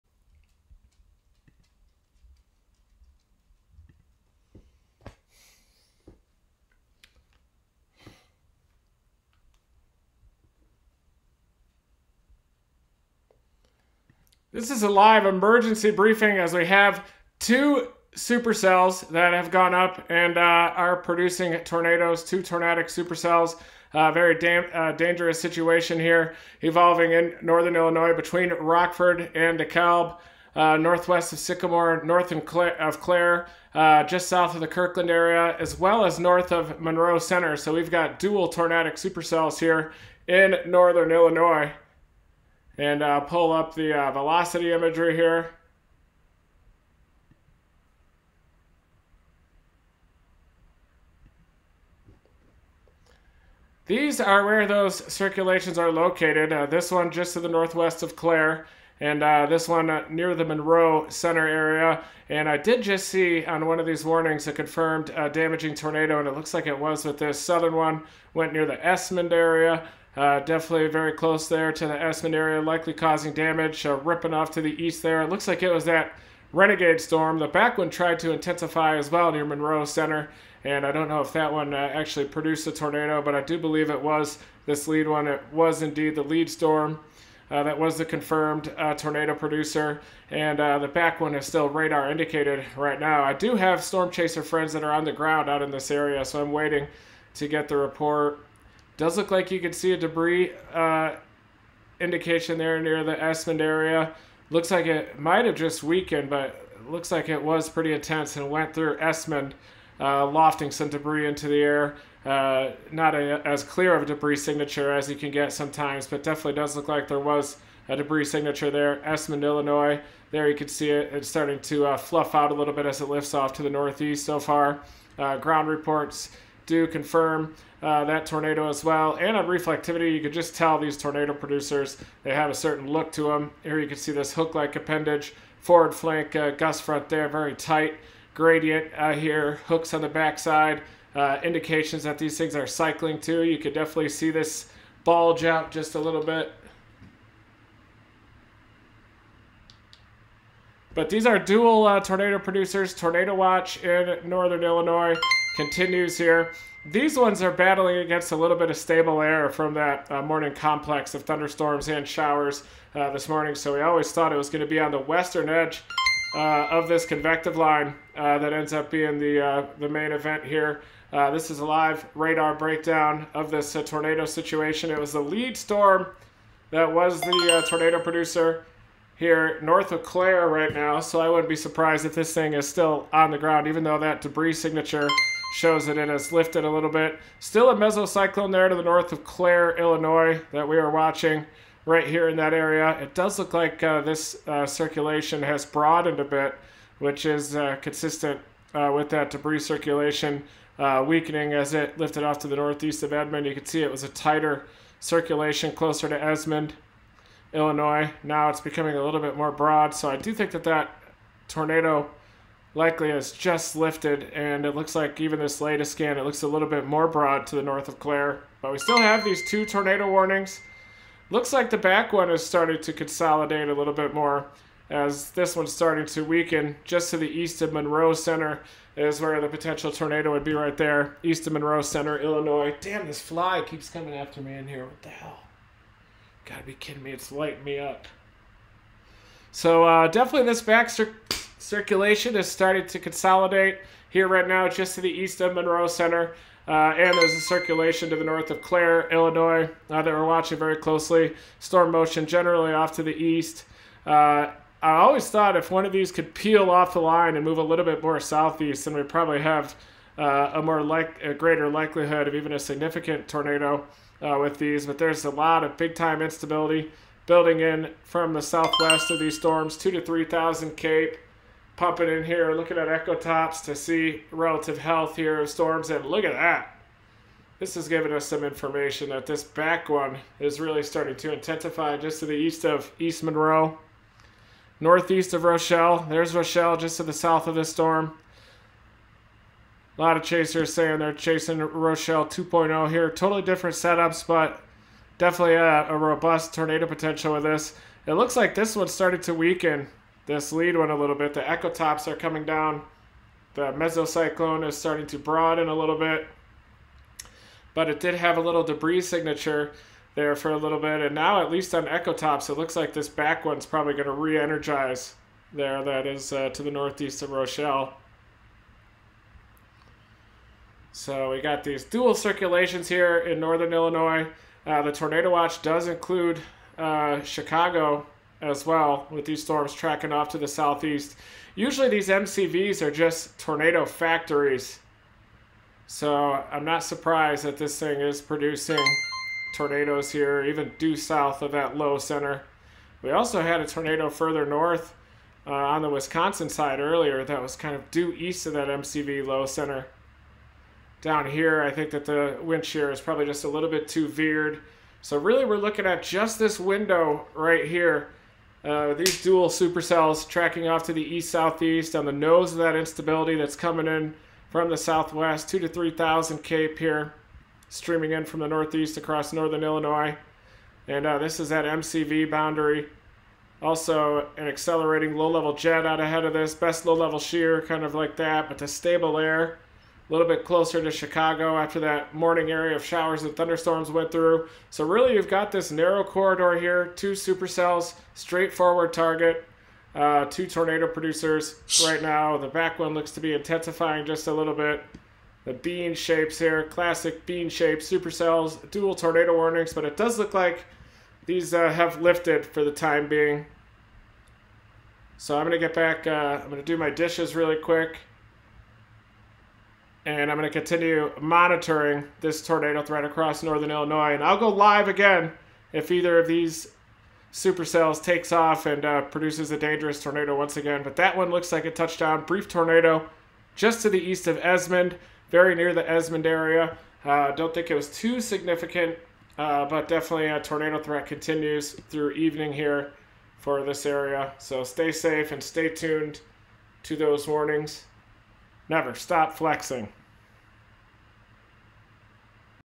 LIVE radar breakdown of tornado threat in northern Illinois! Brief tornado happened near Esmond!
Radar breakdown of the ongoing tornado threat in northern Illinois! A tornado watch is in effect through evening for northern Illinois including Chicago, extending into northwest IN. There are two tornado warned supercells in progress right now!